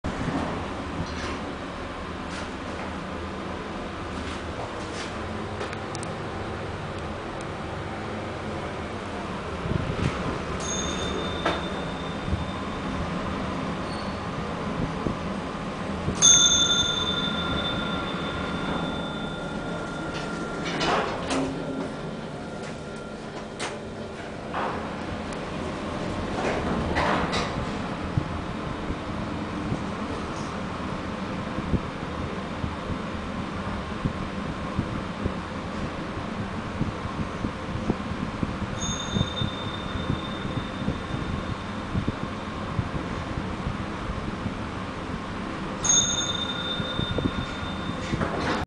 Liang Court, Singapore - Hitachi Buil-ACE Astro B79 Freight Elevator (Novotel Wing)
Technical specifications :
Floor served : B1, 1 to 5, 8
Capacity : 13 persons/900kg
Manufacturer : Hitachi Elevator 日立
Model: Buil-ACE Astro B79 ビルエースアストロ
Year installed: 1983
History : Original